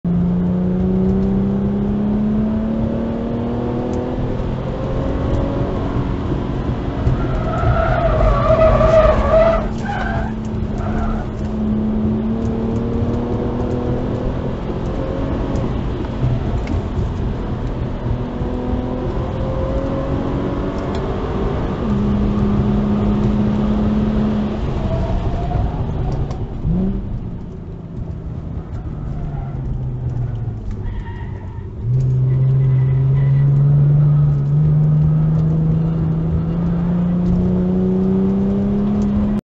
How road tires react to racing 😱
Lapping incident at ICAR with 2008 G37S. Rubber is Antares Ingens A1 budget tires.